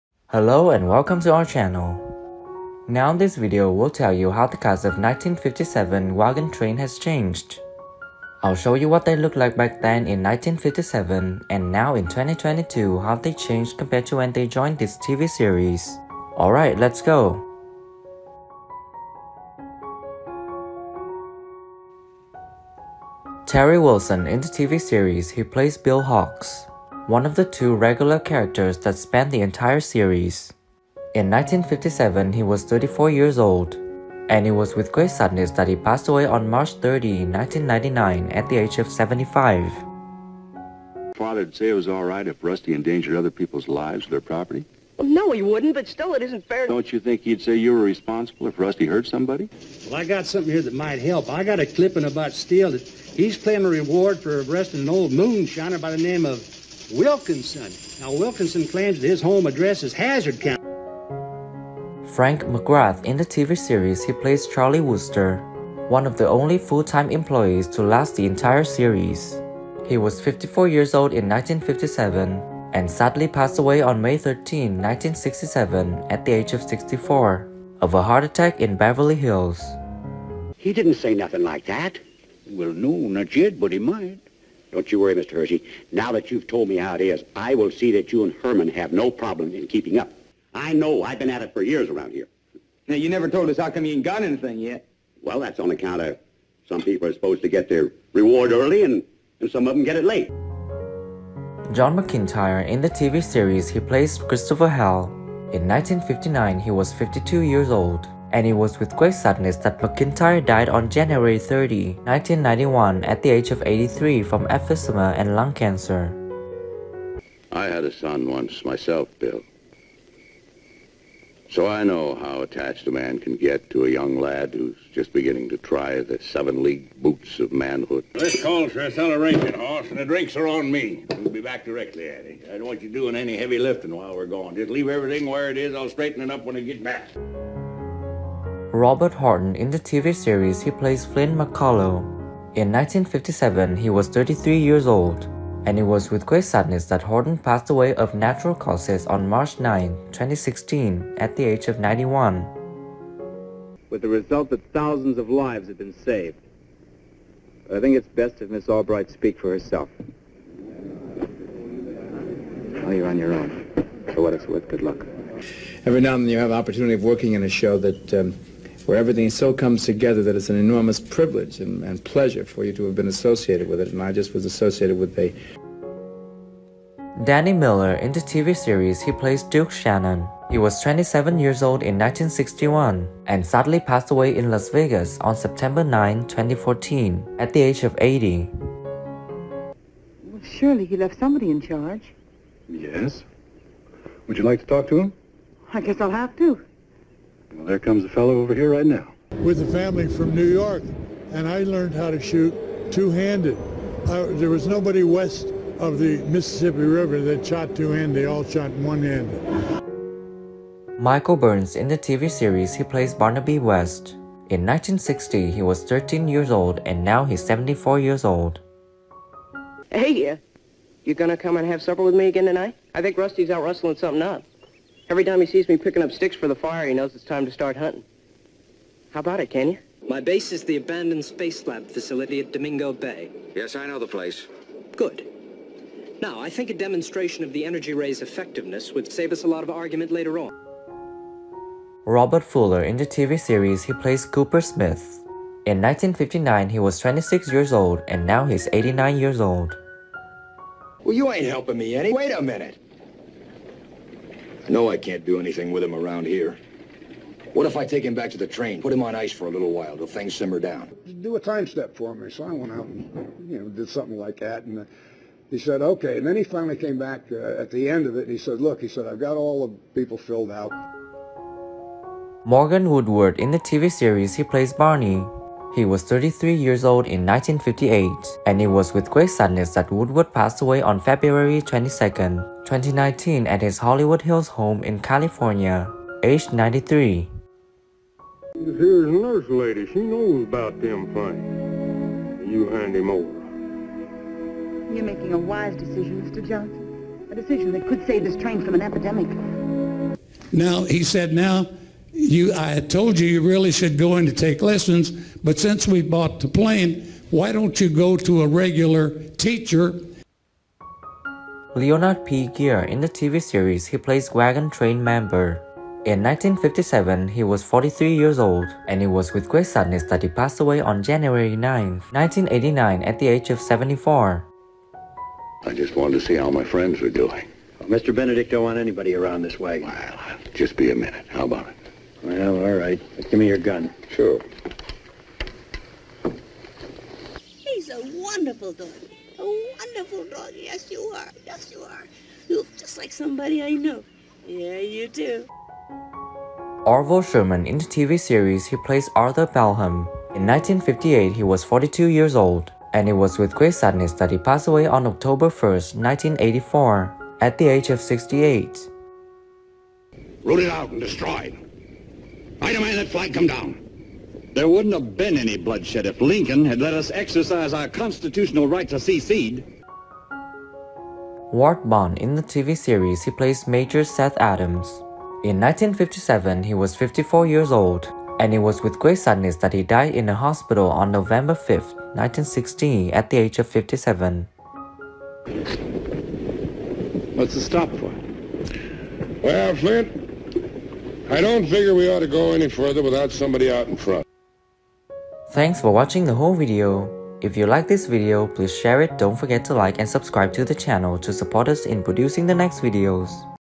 WAGON TRAIN 1957 Cast: THEN & NOW 2022 Thanks For The Memories
Wagon Train is an American Western series that aired 8 seasons: first on the NBC television network (1957–1962), and then on ABC (1962–1965). Wagon Train debuted on September 18, 1957, and became number one in the Nielsen ratings. It is the fictional adventure story of a large westbound wagon train through the American old West, from Missouri to California. Its format attracted different famous guest stars per episode, as travelers or as residents of the settlements they encountered. The show initially starred supporting film actor Ward Bond as the wagon master (replaced after his death in 1960 by John McIntire) and Robert Horton as the scout (eventually replaced by similar-looking Robert Fuller when Horton opted to leave the series).